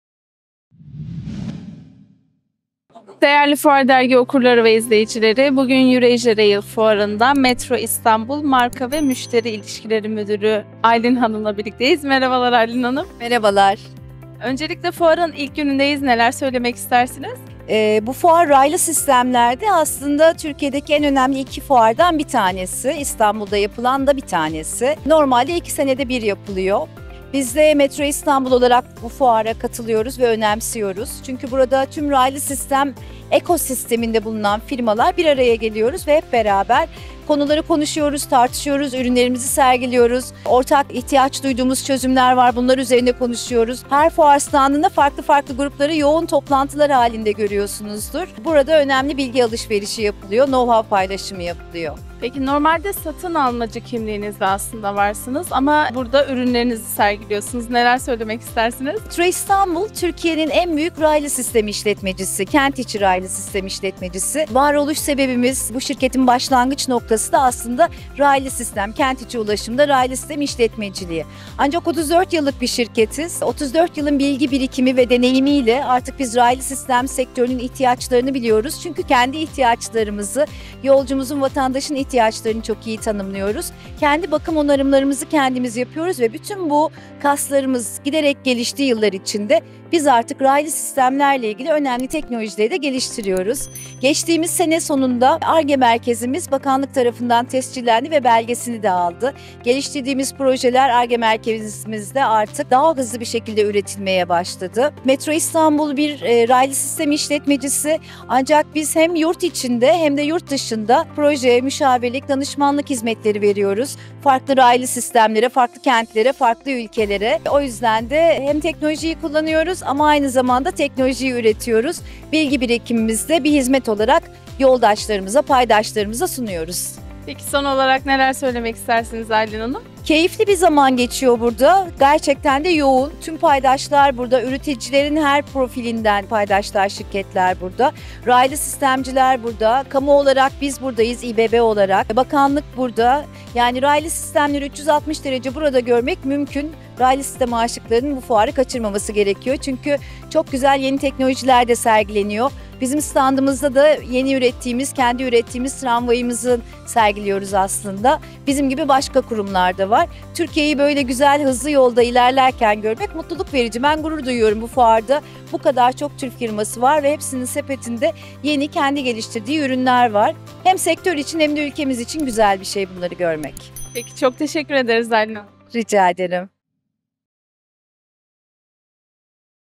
Fuar Dergisi TV-Eurasıarail-Metro İstanbul Marka ve Müşteri İlişkileri Müdürü Aylin Erol
Metro İstanbul Marka ve Müşteri İlişkileri Müdürü Aylin Erol, Eurasiarail 2023 Fuarında Fuar Dergisi Mikrofonlarımıza Özel Açıklamalarda Bulundu.